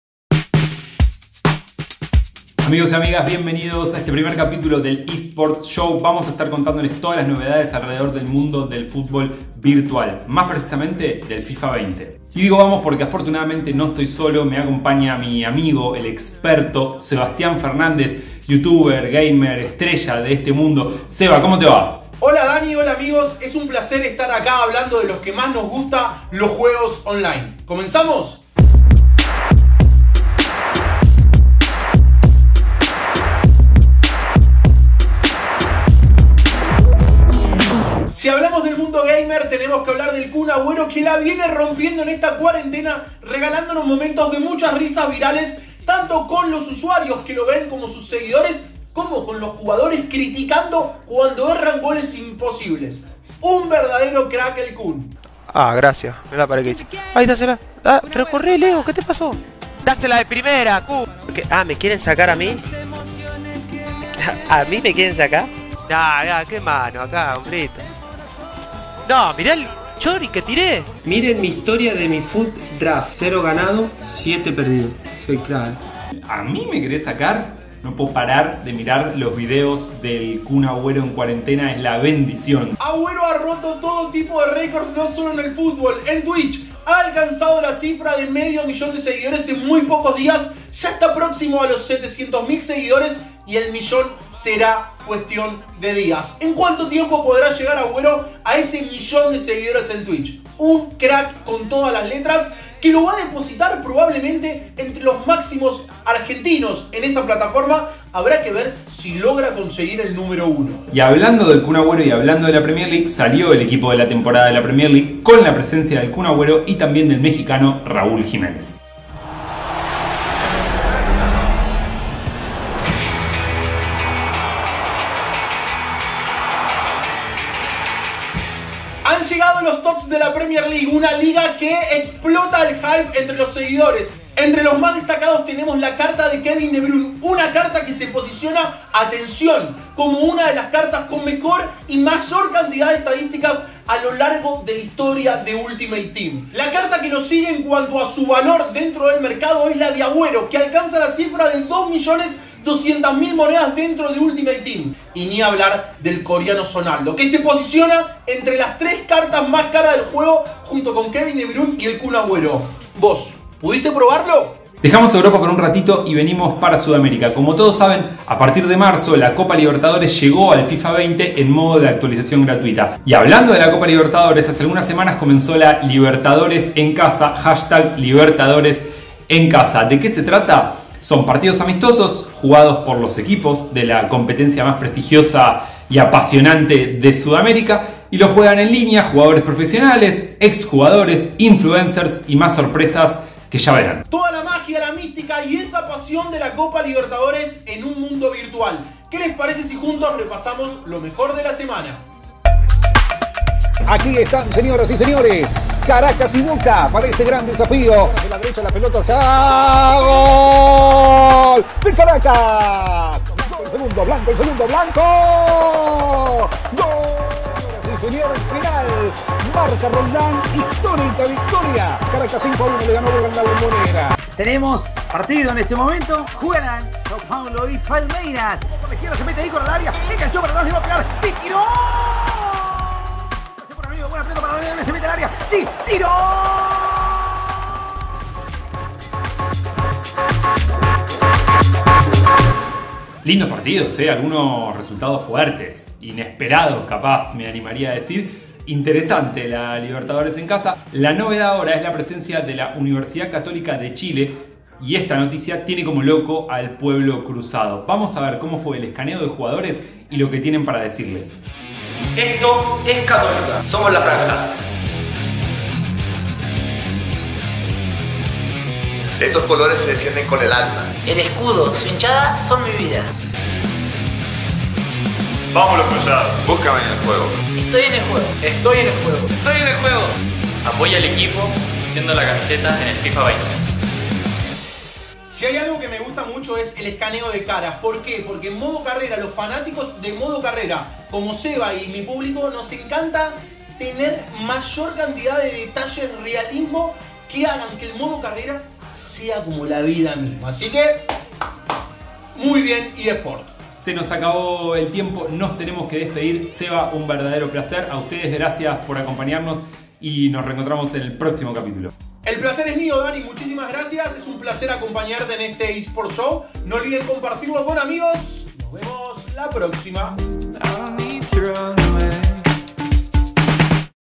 KUN AGÜERO, EL REY STREAMER 💥 eSports Show E01
¡Las últimas novedades del FIFA20 y el fútbol virtual! 🎬 🎮 El último estreno de EA Sports , con Sebas Fernández y Daniel Gabin